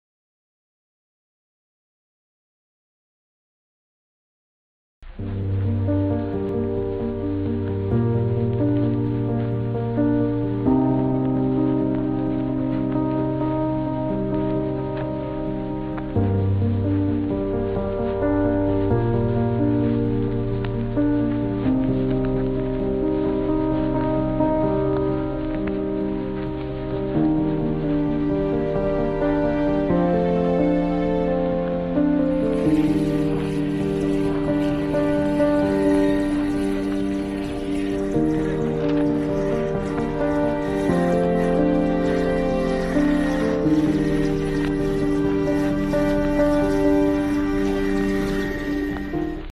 At least 920 killed in Afghanistan 6.0 magnitude earthquake | ASD News
920 killed, 600 injured in 6.1 magnitude earthquake in eastern Afghanistan